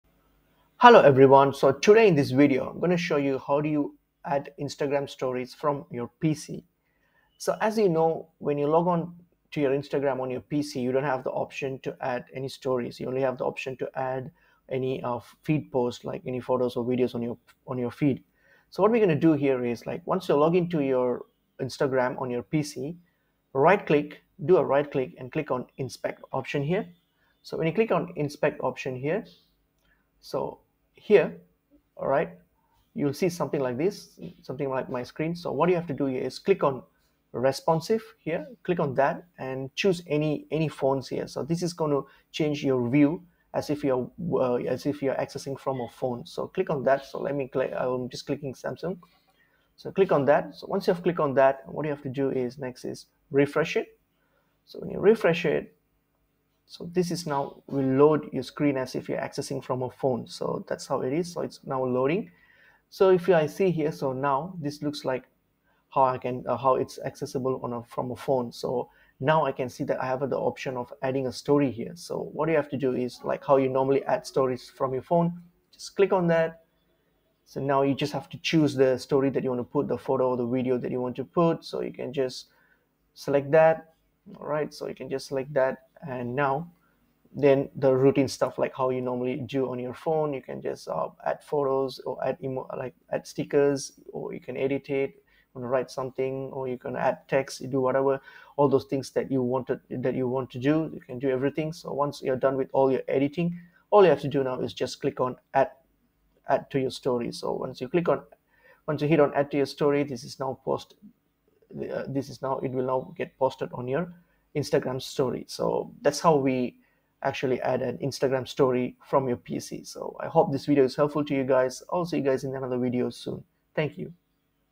How to Upload Instagram Stories on Laptop PC
How to Upload Instagram Stories on PC. In this video, we're going to show you how to upload Instagram stories on a laptop computer.

Uploading stories on Instagram from your laptop is simple and easy, and this video will teach you how to do it in just a few minutes. After watching this video on How to Upload Instagram Stories on PC, you'll be able to upload stories to instagram from your laptop like a pro!
How to Upload Instagram Stories on PC